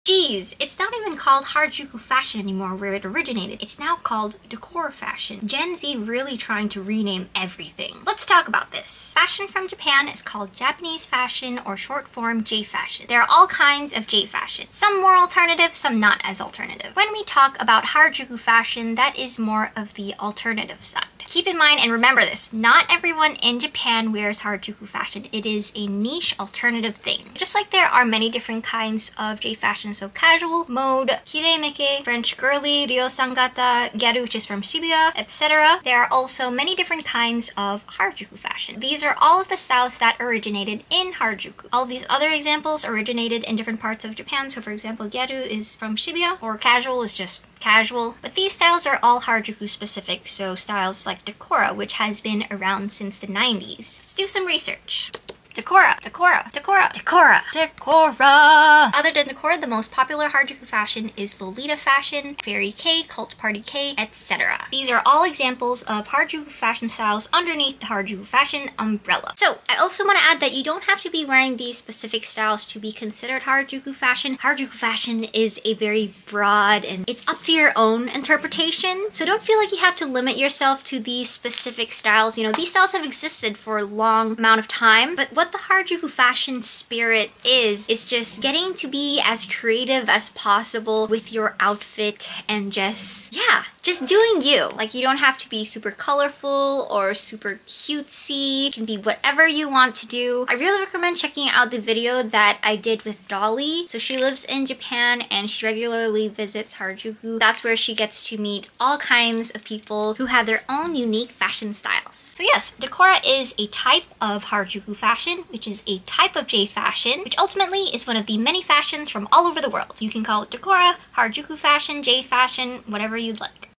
Harajuku vs Decora: What Is The Difference?
What Is It Like To Be a Harajuku Kid in Japan? Interview With Dolly a.k.a That Rainbow Chick